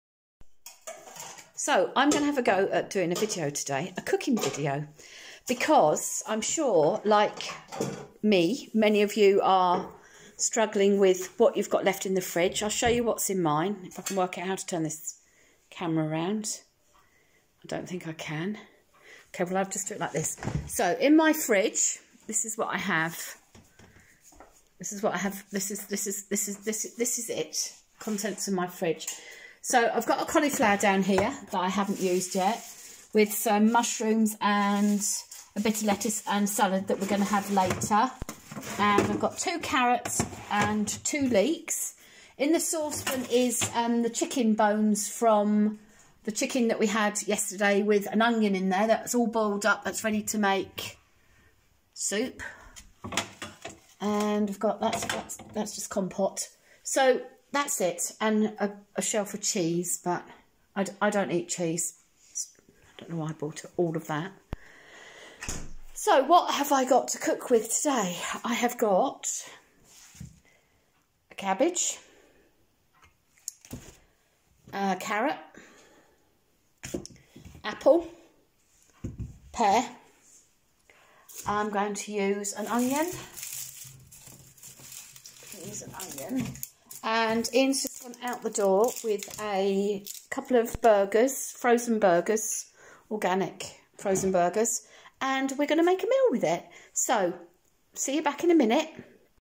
First video cooking with Love and what is left in the cupboards. Fruity salad today.
Part 1 of 3. Very first video.
As we all try and stay at home for as long as possible before heading to the shops, I'm here to show you what I'm doing with what I have left to make low cost healthy meals.  Real and authentic, this is cooking with love.